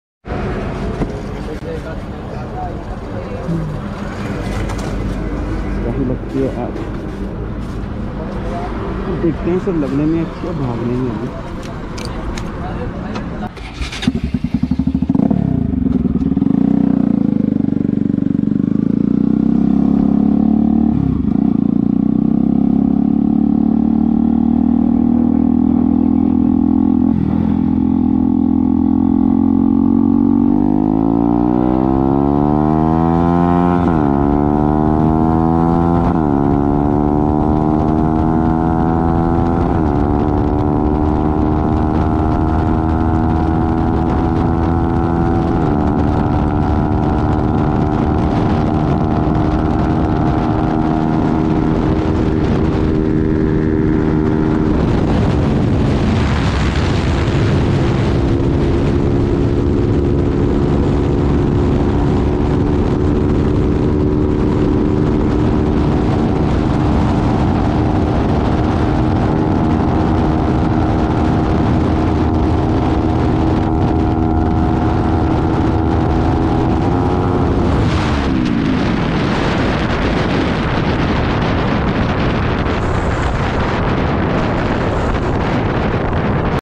Ns200 Bs4 Adrenaline rush |Modified Ns200 | Top end test
Today i have brought you guys the video of full Adrenaline boost of ns 200 bs4 variant